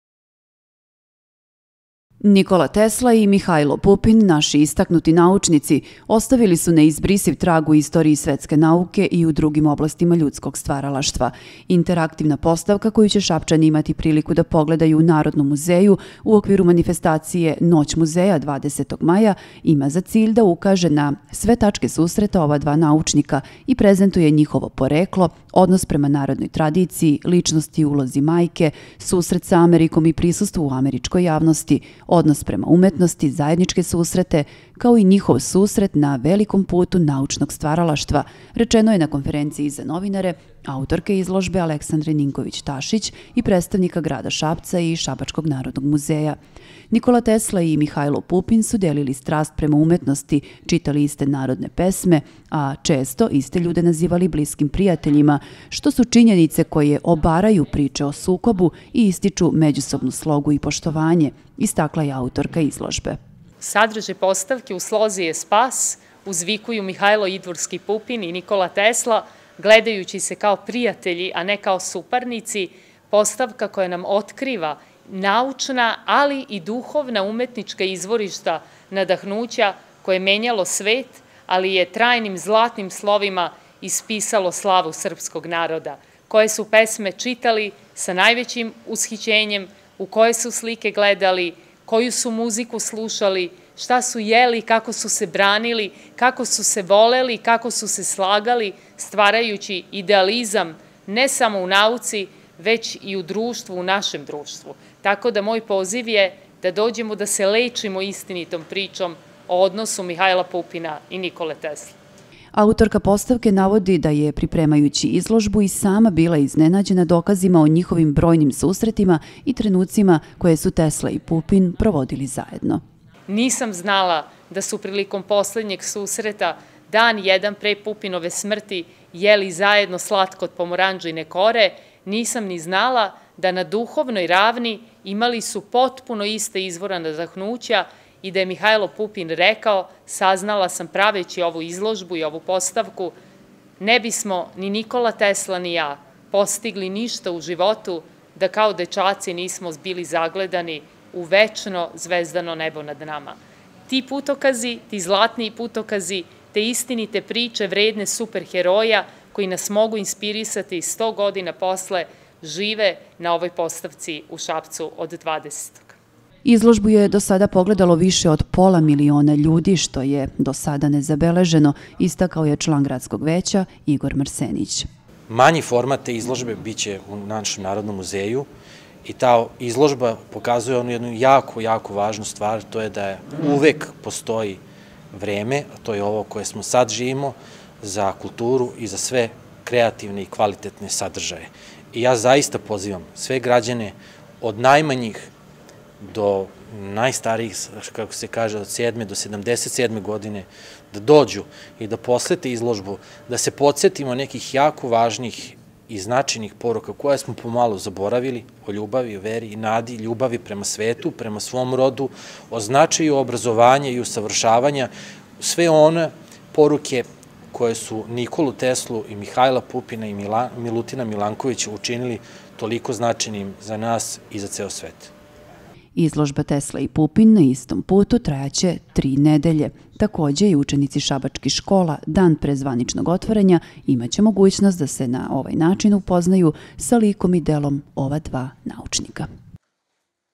Izložba "Tesla i Pupin na istom putu" u Šapcu od 20. maja
Izlozba pod nazivom „Tesla i Pupin na istom putu“ bice priredjena u Narodnom muzeju u Sapcu u okviru „Noci muzeja“ 20. maja. Sve do 15. juna Sapcani ce imati priliku da pogledaju postavku autorke Aleksandre Ninkovic Tasic koja govori o tome gde su se nasi velikani sretali, sta su zaista jedan o drugome govirili i kako su matematicki saradjivali. Nikola Tesla i Mihajlo Pupin su nasi istaknuti naucnici koji su ostavili neizbrisiv trag u istoriji svetske nauke, ali i u drugim oblastima ljudskog stvaralastva. Nakon sastanka direktora obrazovnih ustanova u Sapcu i organizatora postavke, na konferenciji za novinare naglaseno je da ce izlozbu imati priliku da pogledaju i sabacki ucenici i to dan ranije, 19. maja.